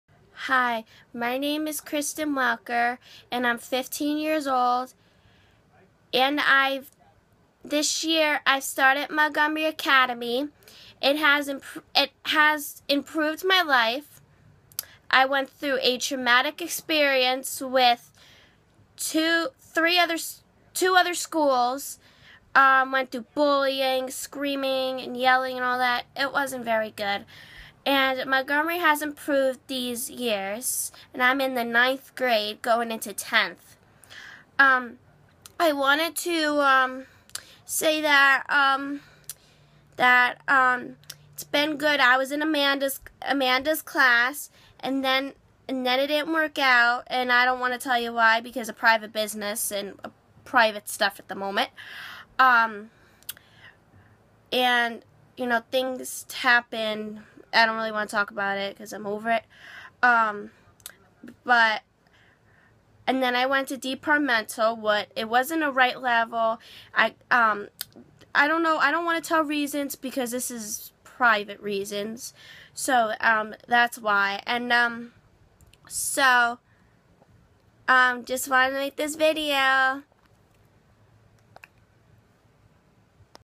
Montgomery Academy
Hi my name is Kristen Welker I am 15 years old  l  going into 10th grade Montgomery has been a good school I got kicked out of  New Road  school and  Hillsborough high  school I have been in  Hillsborough Schools since I was three years old